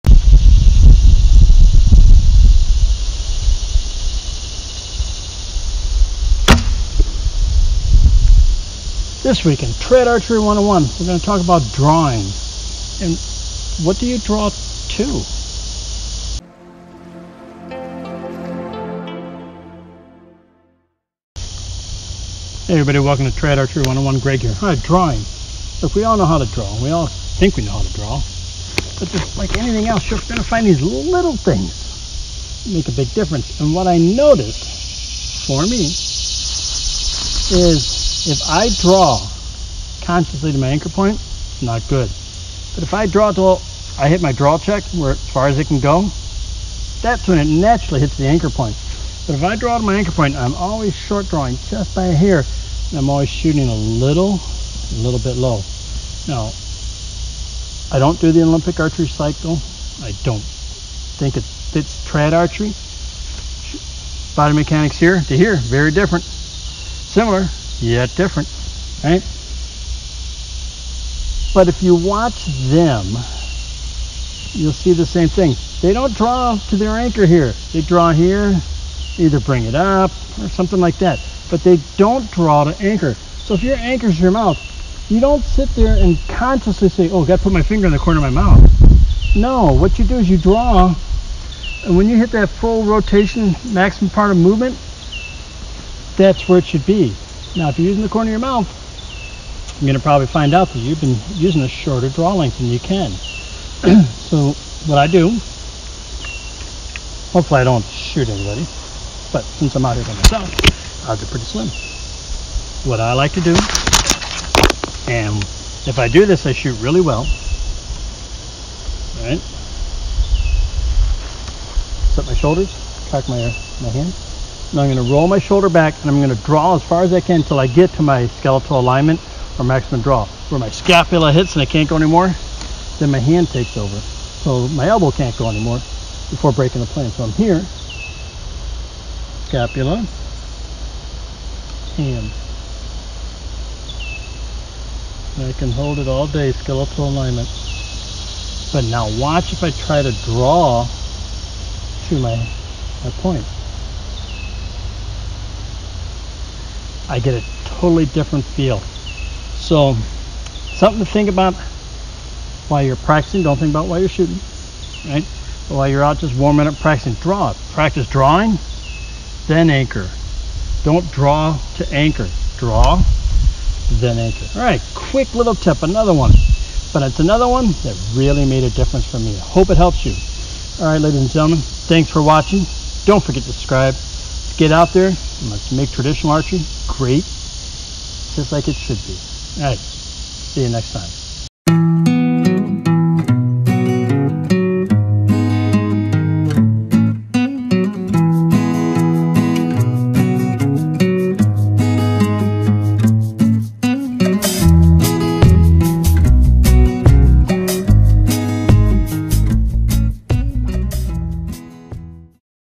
A Quick Point About Your Draw
A Quick Point about drawing. Should you draw to your anchor?